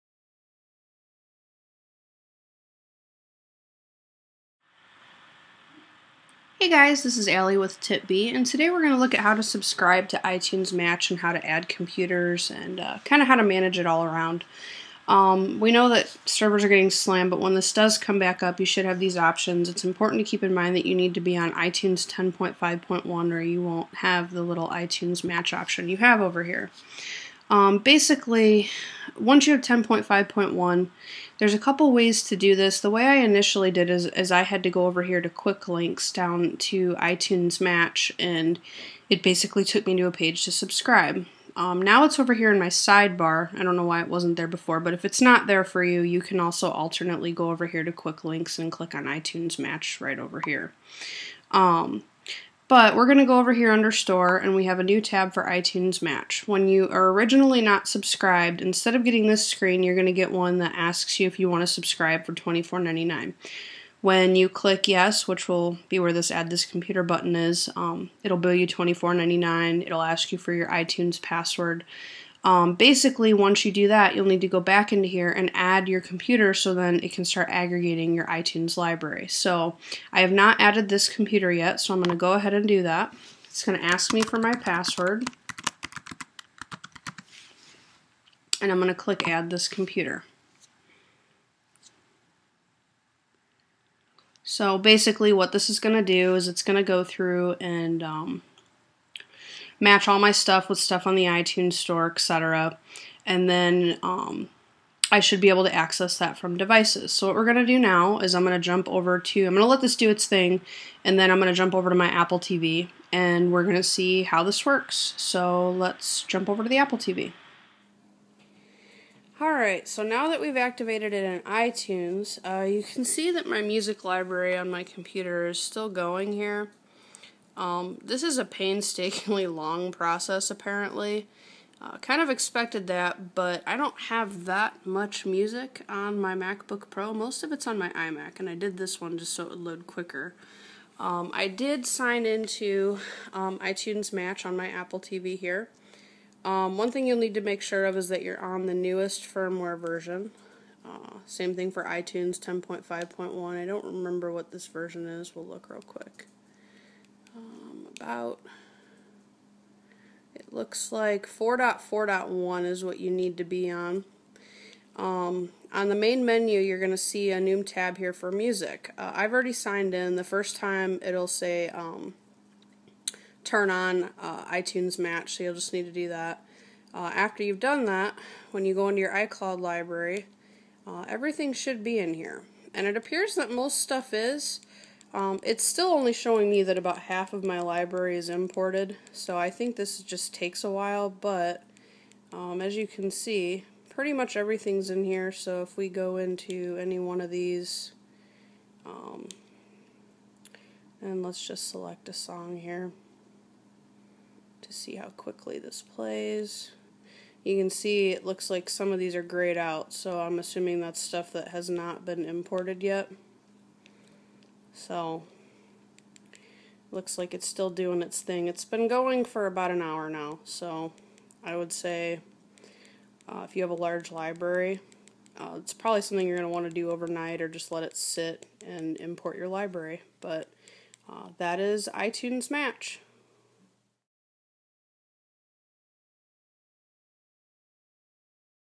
How to subscribe to iTunes Match
We then take a look at how iTunes Match performs on the AppleTV 2nd generation.